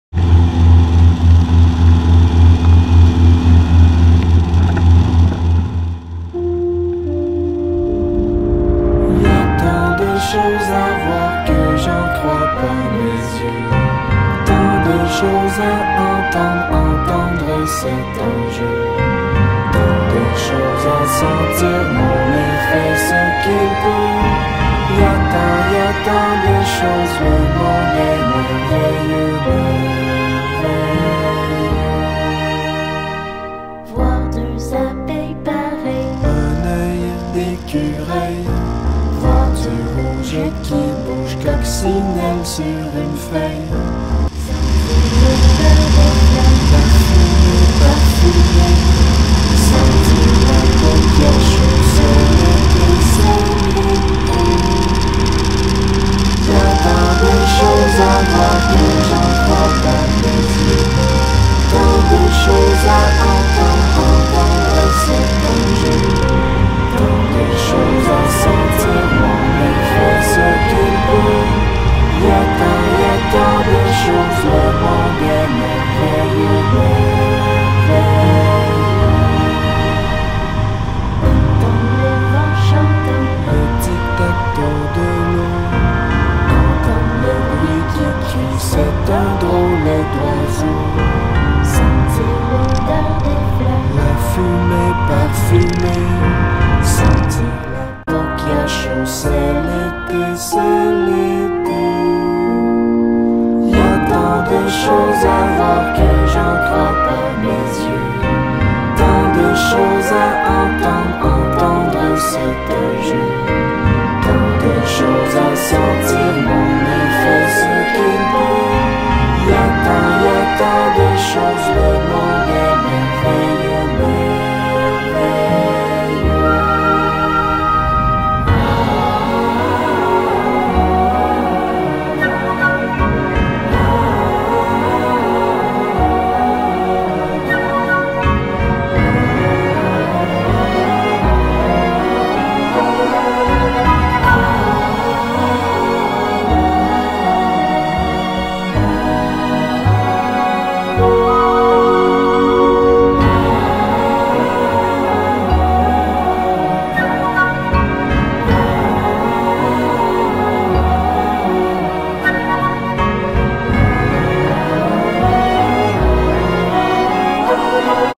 Vol-voyage camping avec mon fils - Casey 2017
Un montage vidéo et photos de mon vol-voyage de camping en septembre dernier avec mon fils de 13 ans. De bons souvenirs créés ensemble. Un superbe endroit pour aller camper.